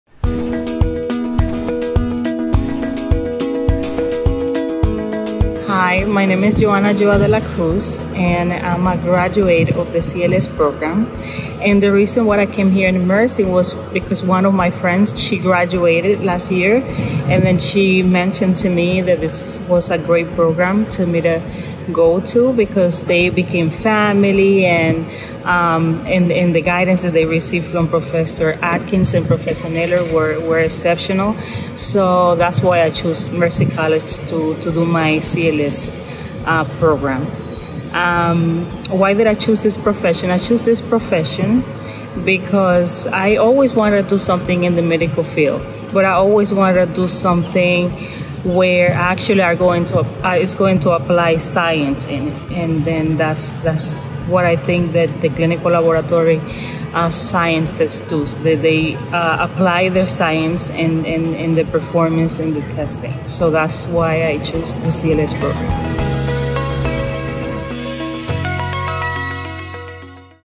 Meet Johanna Joa Delacruz ('19), Student in Mercy's Clinical Lab Science Program
Clinical Lab Science graduate Johanna Joa Delacruz on why she chose the CLS program at Mercy and what her job prospects are now.